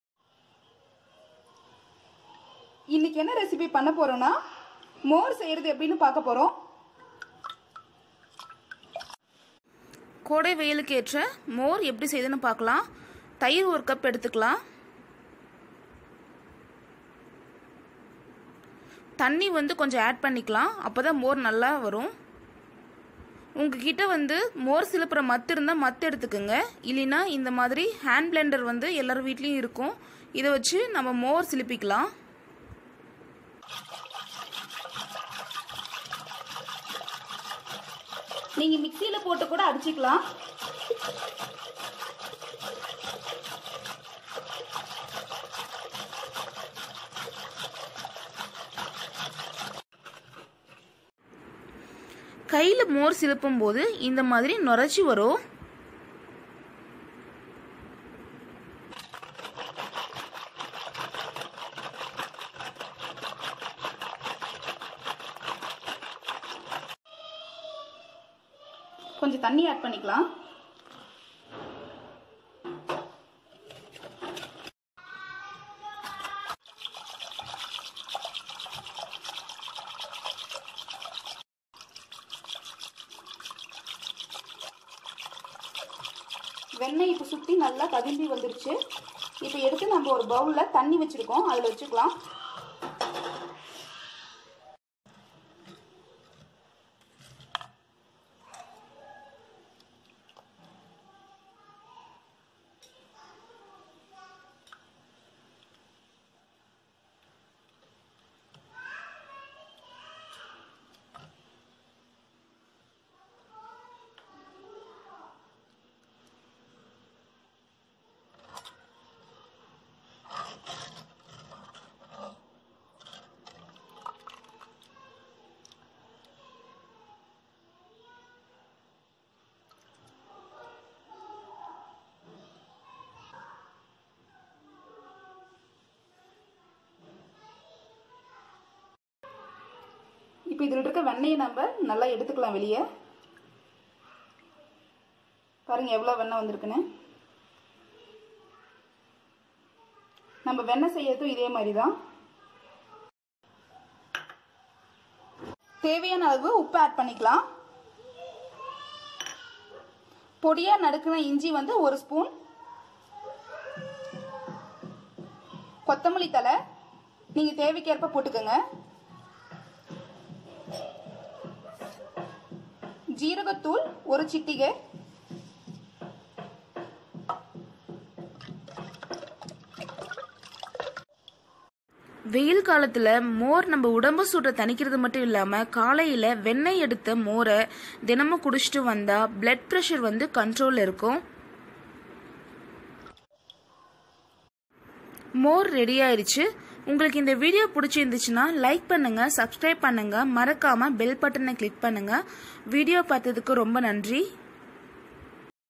Butter Milk Recipe | Neer Moor | மோர் செய்வது எப்படி??
Butter Milk Recipe | Neer Mor | மோர் செய்வது எப்படி??

Ingredients

Ginger - 1 tbs ( chopped)
Jeera powder - 1 pinch
Coriander leaves - chopped
Salt
Water Tamil samayal videos, Cooking Video in Tamil, Tamil cooking Video,